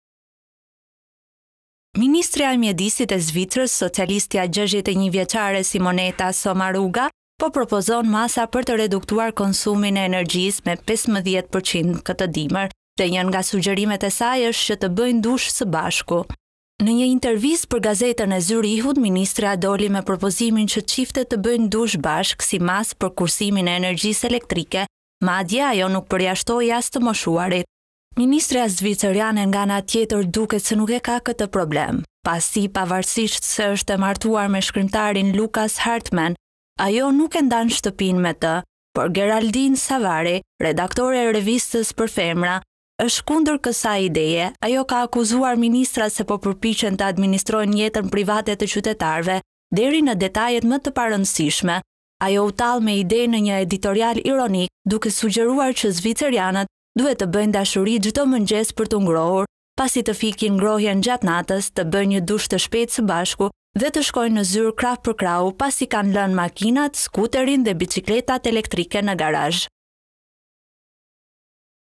Top News - ‘Bëni dush bashkë për të kursyer’. Ministrja zviceriane ‘habit’ me këshillat anti-krizë
Ministrja e mjedisit e Zvicrës, socialistja 61-vjeçare Simonetta Sommaruga, po propozon masa për të reduktuar konsumin e energjisë me 15% këtë dimër dhe një nga sugjerimet e saj është që të bëjnë dush së bashku.
Në një intervistë për gazetën e Zyrihut, ministrja doli me propozimin që çiftet të bëjnë dush bashkë, si masë për kursimin e energjisë elektrike, madje ajo nuk përjashtoi as të moshuarit.
Ministrja zviceriane nga ana tjetër, duket se nuk e ka këtë problem pasi, pavarësisht se është e martuar me shkrimtarin Lukas Hartmann, ajo nuk e ndan shtëpinë me të.
Por, Geraldine Savary, redaktore e revistës për femra Femina, është kundër kësaj ideje. Ajo ka akuzuar ministrat se po përpiqen të “administrojnë jetën private të qytetarëve, deri në detajet më të parëndësishme”.
Ajo u tall me idenë në një editorial ironik, duke sugjeruar që zviceranët duhet të bëjnë dashuri çdo mëngjes për t’u ngrohur pasi të fikin ngrohjen gjatë natës, të bëjnë një dush të shpejtë së bashku dhe të shkojnë në zyrë “krah për krahu, pasi kanë lënë makinat, skuterin dhe biçikletat elektrike në garazh.
Pamje: AFP
Photo Source:Reuters